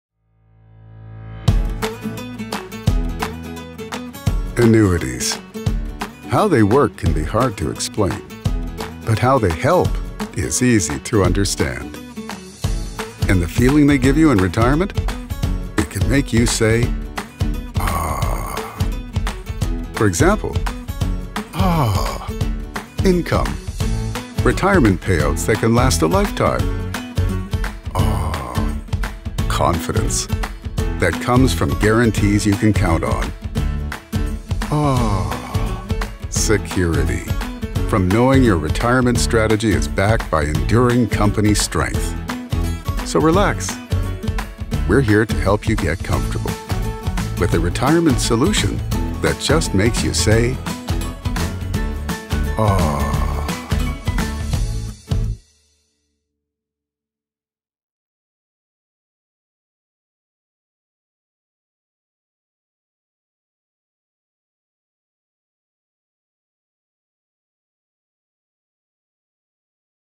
Ahhhnnuities … How Annuities Can Make You Feel
See how annuities can help you relax in retirement and say “Ahhh…”

📚 More Resources!

Integrity Life Insurance Company | National Integrity Life Insurance Company | Western-Southern Life Assurance Company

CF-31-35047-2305